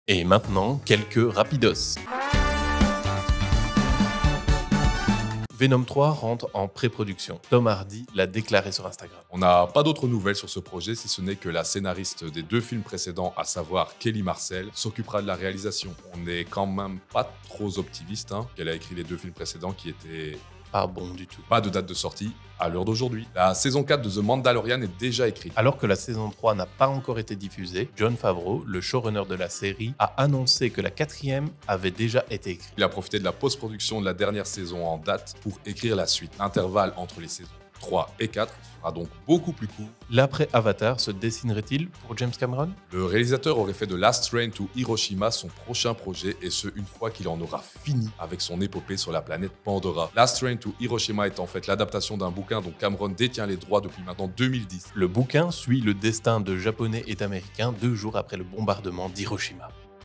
DES NEWS RAPIDOS !
Rapidos

Venom 3 rentre en pré-production. Tom Hardy l’a déclaré sur instagram. Pas d’autres nouvelles sur ce projet, si ce n’est que c’est la scénariste des deux films précédents, à savoir Kelly Marcel, qui va réaliser ce troisième opus. Bon étant donné qu’elle a écrit les deux premiers films, on est pas super optimiste pour ce nouveau film. Pas de date de sortie à l’heure d’aujourd’hui.

La saison 4 de The Mandalorian déjà écrite. Alors que la saison 3 n’est pas encore diffusée, Jon Favreau, le showrunner de la série, a annoncé que la 4e saison est déjà écrite. En effet, il a profité de la post-production de la dernière saison en date pour écrire la suite. L’intervalle entre les saisons 3 et 4 sera donc beaucoup plus court. La saison 3 de The Mandalorian sera diffusée sur Disney+ à partir du 1er mars.

L’après-Avatar se dessinerait-il pour James Cameron ? Le réalisateur aurait fait de Last Train To Hiroshima son prochain projet, et ce une fois qu’il en aurait finit avec son épopée sur la planète Pandora. Last Train to Hiroshima est l’adaptation d’un bouquin dont Cameron détient les droits depuis maintenant 2010. Le bouquin suit le destin de japonais et d’ américains deux jours après le bombardement d’Hiroshima.